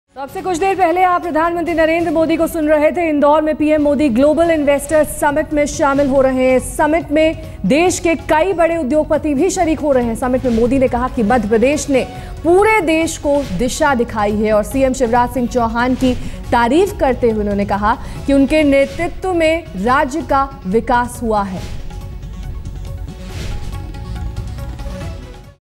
Global Investors' Summit: PM Modi praises MP CM Shivraj Chauhan
At the Global Investors' Summit in Indore, Prime Minister Narendra Modi on Thursday praised Chief Minister Shivraj Singh Chauhan, saying that Madhya Pradesh has made leaps of progress under his leadership.